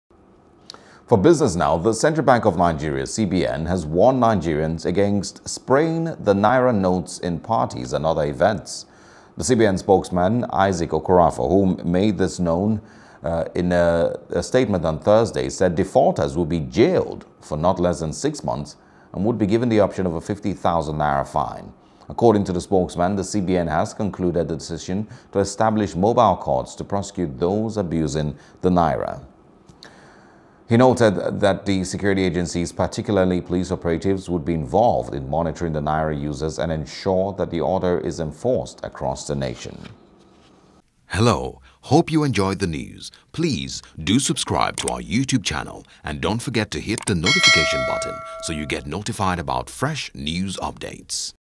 Defacement: CBN Warns Nigerians Against Spraying Naira Notes | BUSINESS
The Central Bank of Nigeria, CBN, has warned Nigerians against spraying the Naira notes in parties and other events.

The CBN spokesman, Isaac Okorafor, who made this known on Thursday in a statement, said defaulters would be jailed for not less than six months and would be given the option of a N50,000 fine.